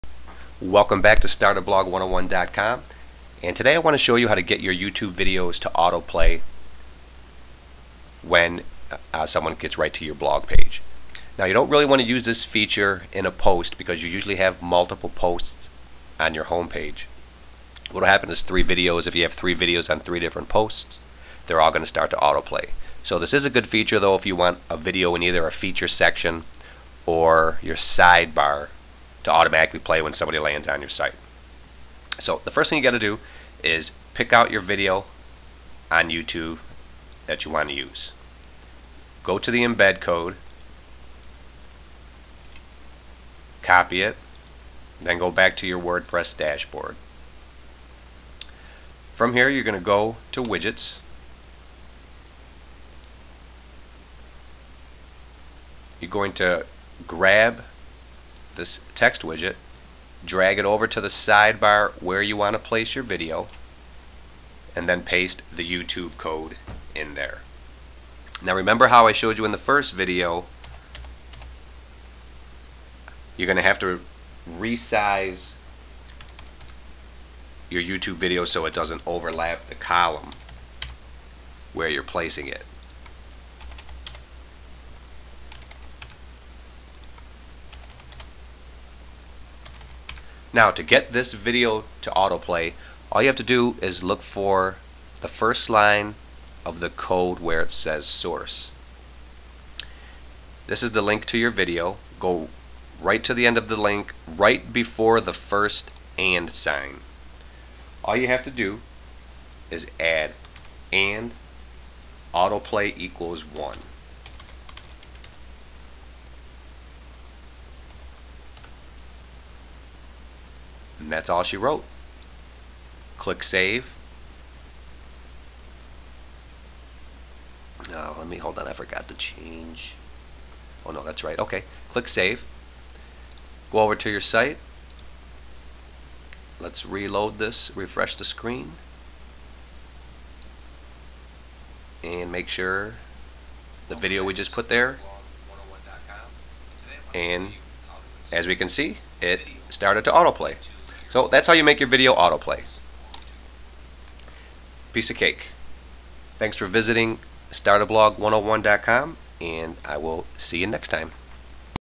How to Make Your Embedded YouTube Video Autoplay
How to make your embedded youtube video autoplay once you place in in your side bar of your WordPress blog or feature section.